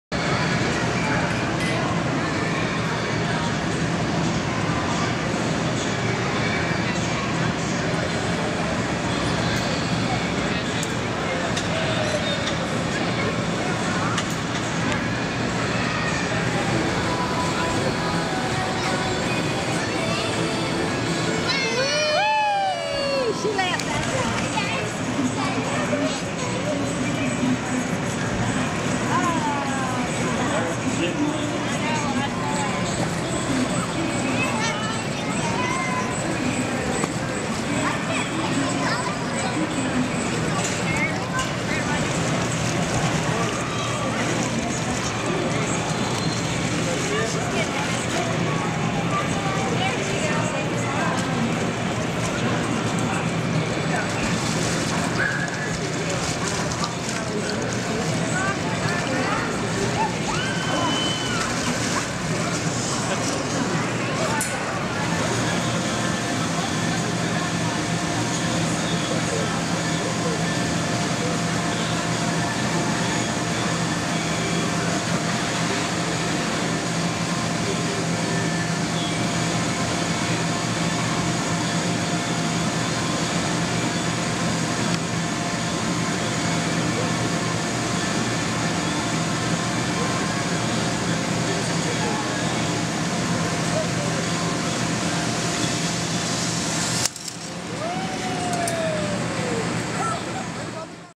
Audrey at Pikeville Kids Fest
We visited Patrick and Bethany in Pikeville this weekend. We took the kids to Kids Fest.